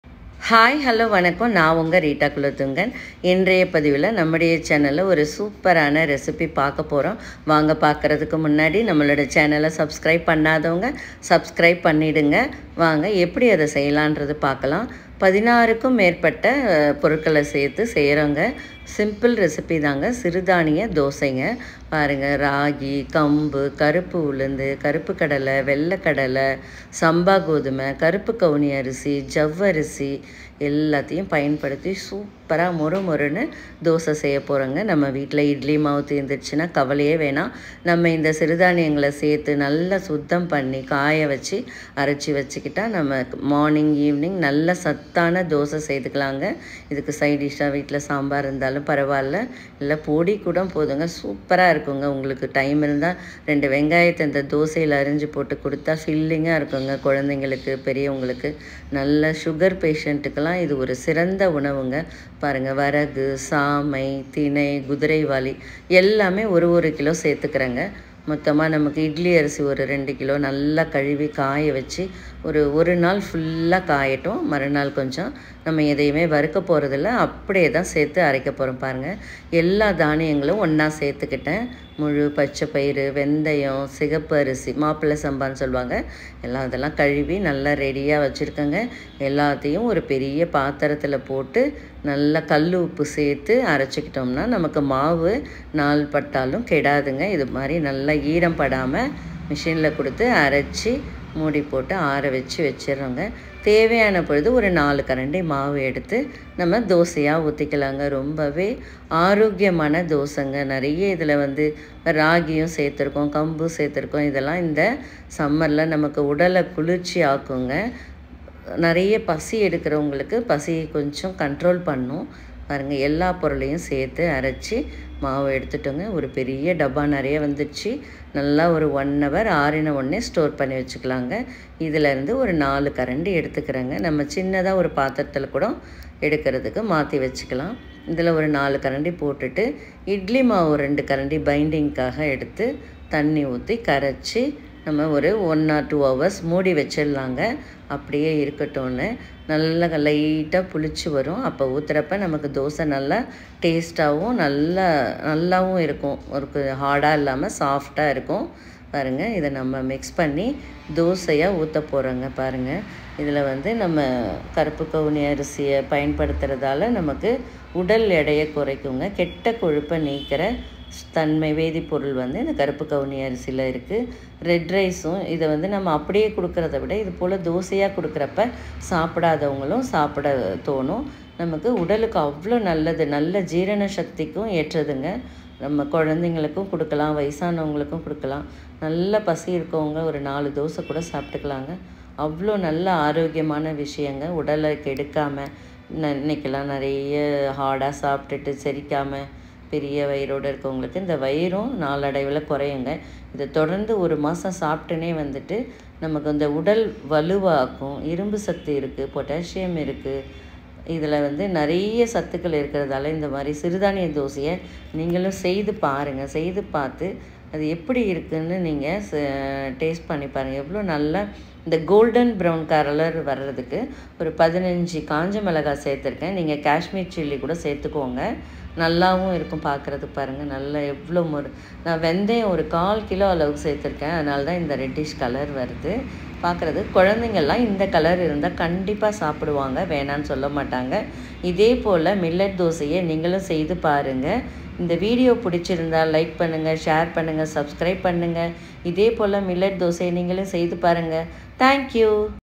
Millets Dosa recipe healthy dosa best diet#பாரம்பரிய_உணவு சிறுதானிய தோசைHigh fiber .iron
millets milletsdosarecipeforweightloss best healthy diet#types of millets dosa recipe#பாரம்பரியஉணவு பாரம்பரிய சிறுதானிய தோசை
தேவையான பொருட்கள்
இராகி-500 கிராம் ,கம்பு-500 கிராம்
கருப்பு உளுந்து-500கிராம் ,மிளகு-50 கிராம்
சீரகம்-50 கிராம் ,மிளகாய்-15
கருப்பு கவுனி அரிசி -250-கிராம்
சம்பா கோதுமை -250 கிராம்
கருப்பு கடலை-250 கிராம்
வெல்ல கடலை-50 கிராம்
ஜவ்வரிசி-50 கிராம் இட்லி அரிசி-2 கிலோ
சிவப்பு அரிசி-1 கிலோ
வெந்தயம்-250 கிராம்,பச்சை பயறு-250